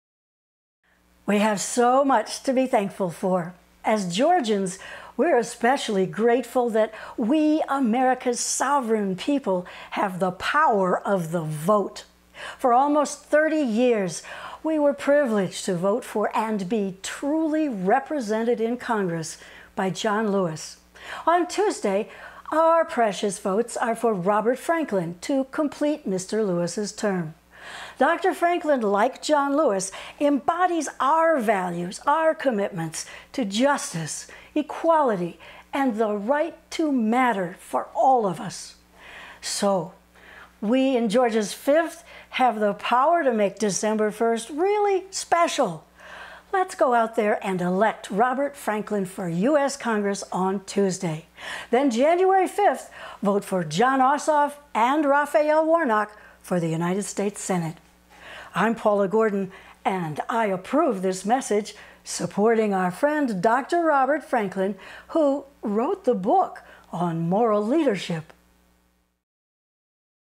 Paula Gordon for Robert M. Franklin
Paula for Franklin for the 5th:
We have SO much for which we’re THANKFUL!!  As Georgians, we’re especially grateful that we, America’s  SOVEREIGN people, hold the POWER of the VOTE.
 For almost 30 years, we were privileged to vote for and be truly represented in Congress by John Lewis.
 On Tuesday, OUR precious votes are for Robert Franklin to complete Mr. Lewis’ term.
 Dr. Franklin -- like John Lewis -- embodies our values, our commitments to JUSTICE, EQUALITY and the right to matter ... for ALL of us.
 SO!...We in Georgia’s FIFTH have the POWER to make  December 1st, really “SPECIAL*”.   Elect Robert Franklin to Congress on TUESDAY!!
. Then, January 5th, vote Jon Ossoff and Raphael Warnock for U.S. Senate!
 I’m Paula Gordon and I approve this message...
...supporting our friend, Dr. Robert Franklin, who “wrote the book” on moral leadership!